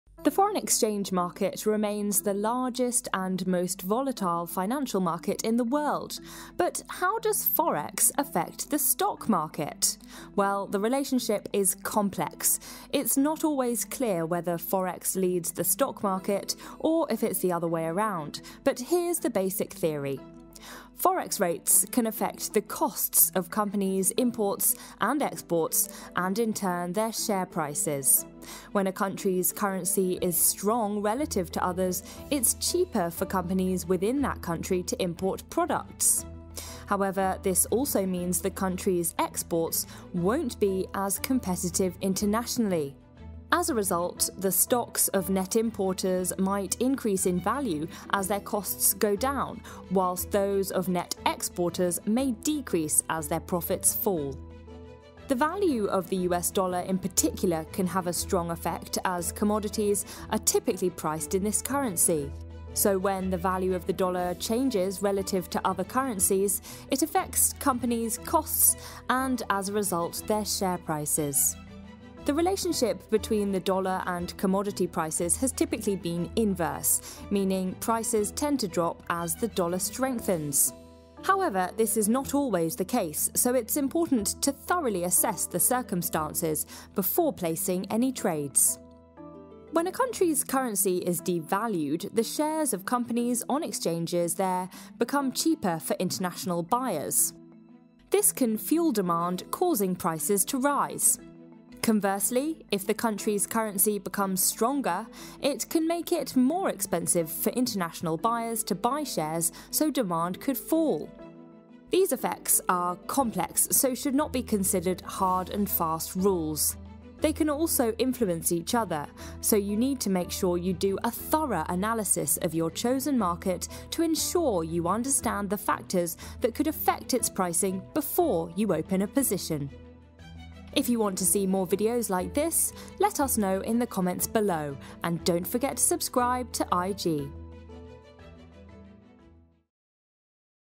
How does forex affect the stock market? | How to trade with IG
Forex and stocks are two of the most traded financial markets in the world, so it is common for traders to look for correlations between the two markets that might help them to predict future price movements. IGTV's Victoria Scholar explains.